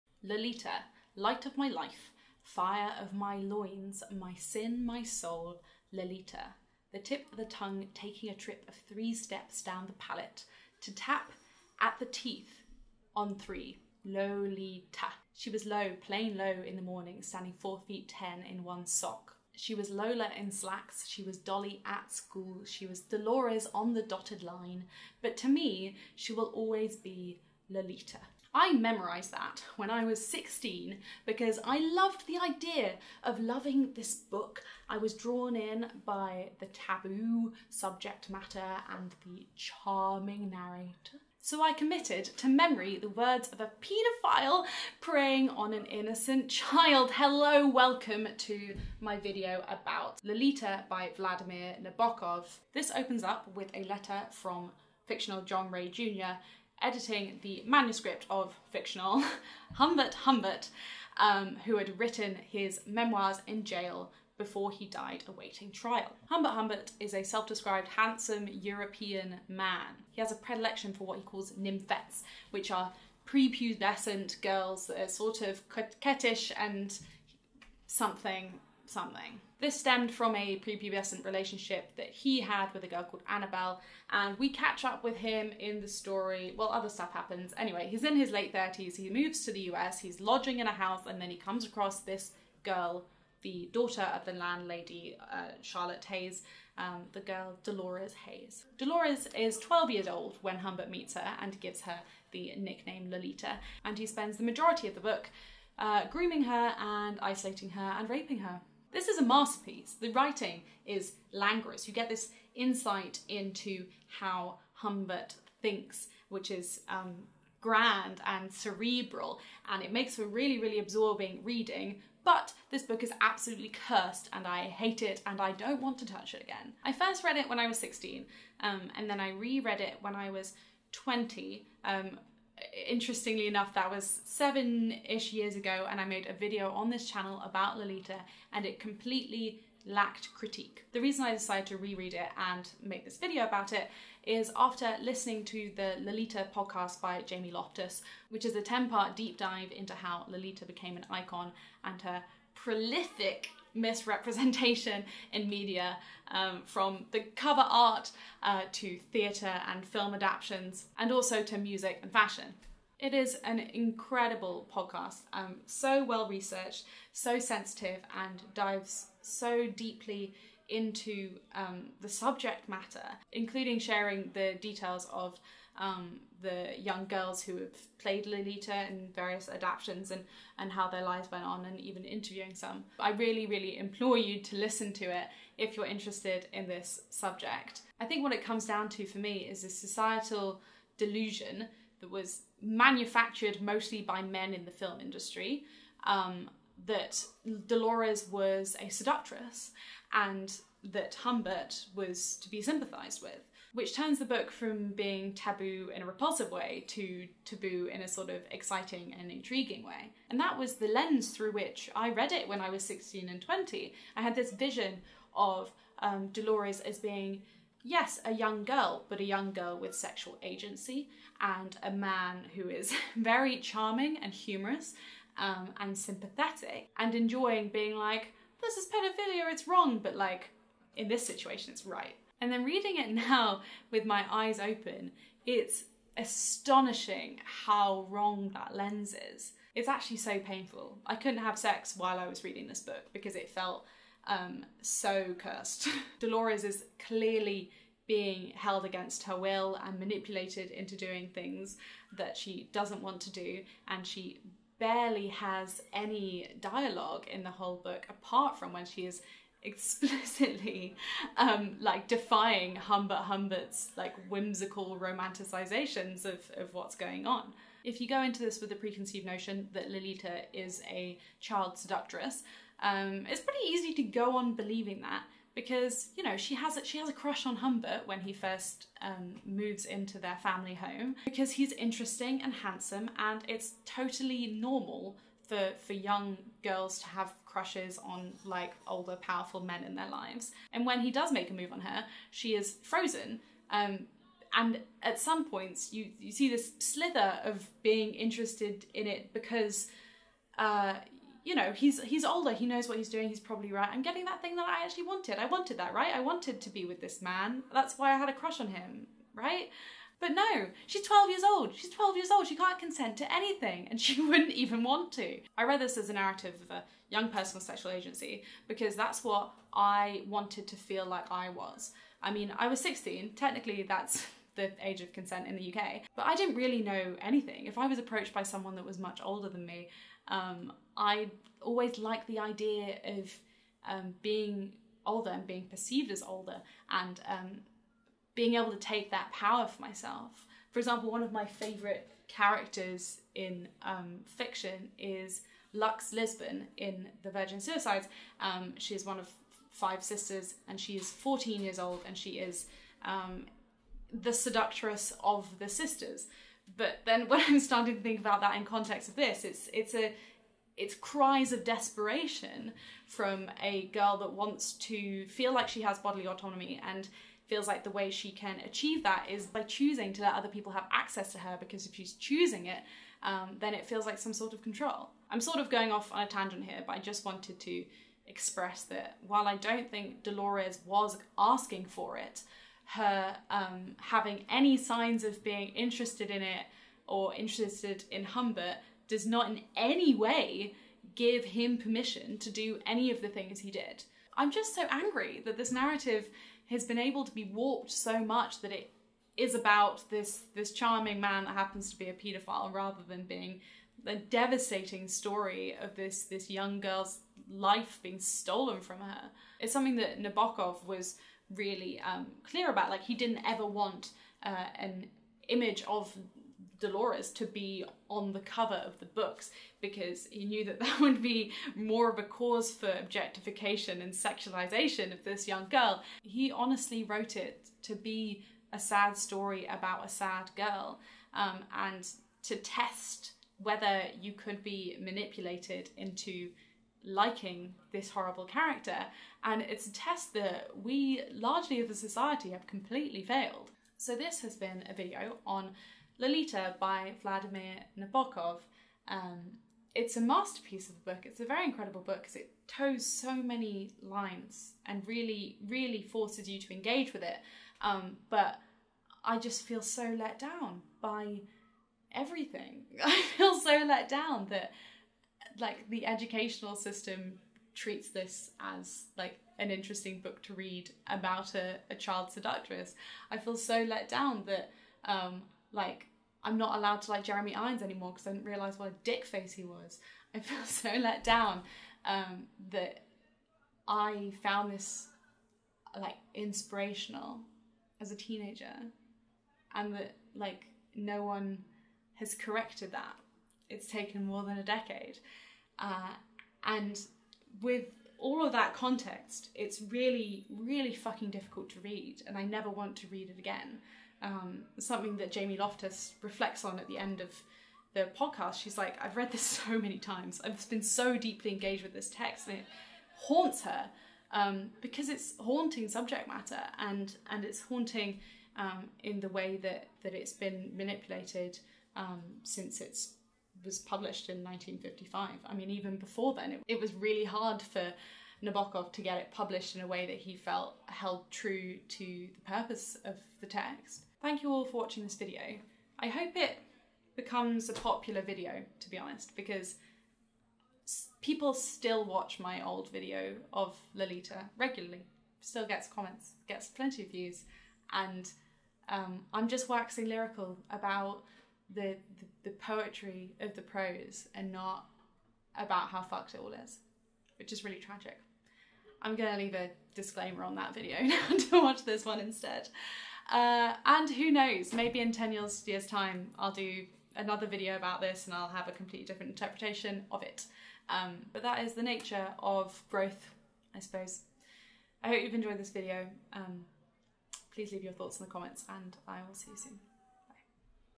Lolita by Vladimir Nabokov
“You see, she had absolutely nowhere else to go.”
Timestamps and links below the fold ↓

0:00 Intro
0:52 Summary
1:49 Review
2:06 Why I re-read it
2:21 Lolita Podcast
3:07 The delusion
4:29 Asking for it
7:24 Angry and sad
8:54 My Lolita legacy